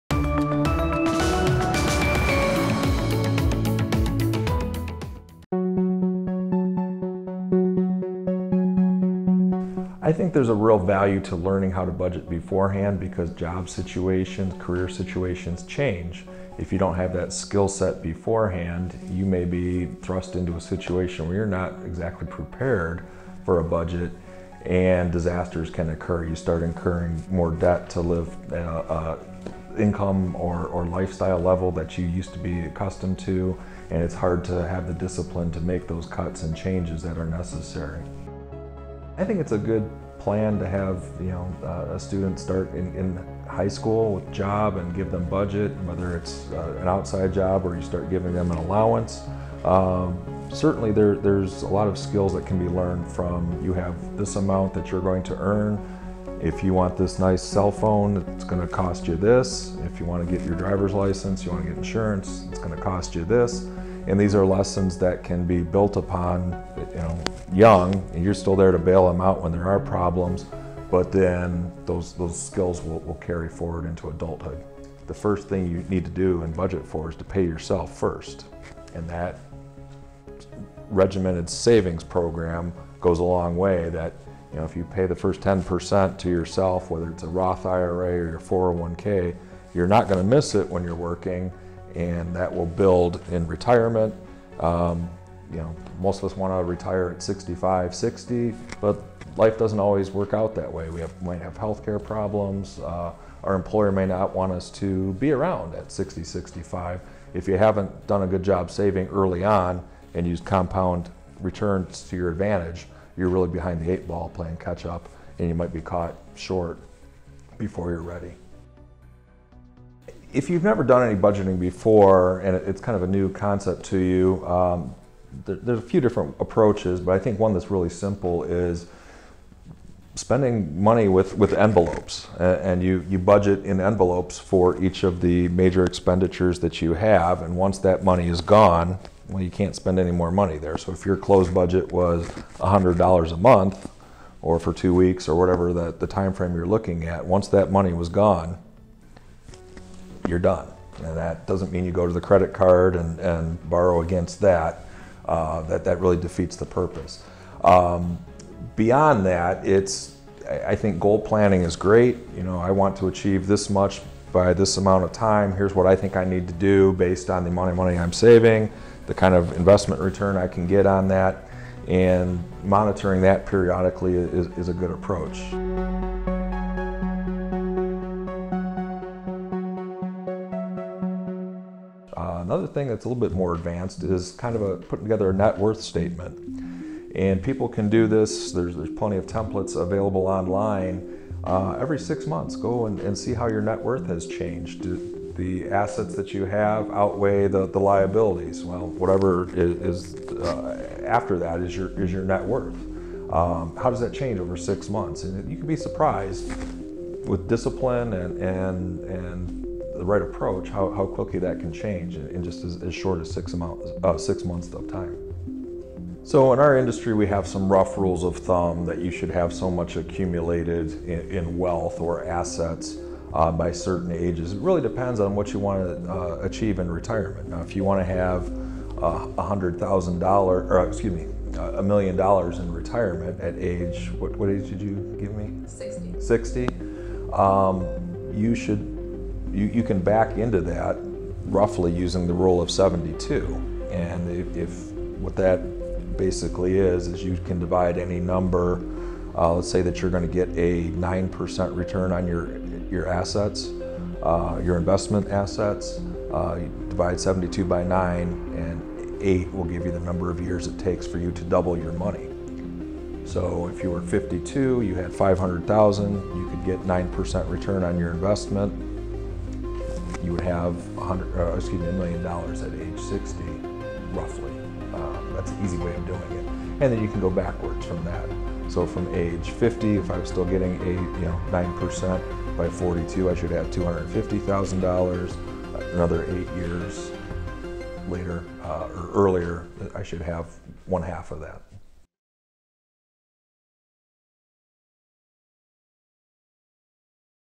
New Year's Resolution - Budgeting
Learn how to better budget your finances in the new year from Perrysburg-based certified financial planner Phillip E. Bollin.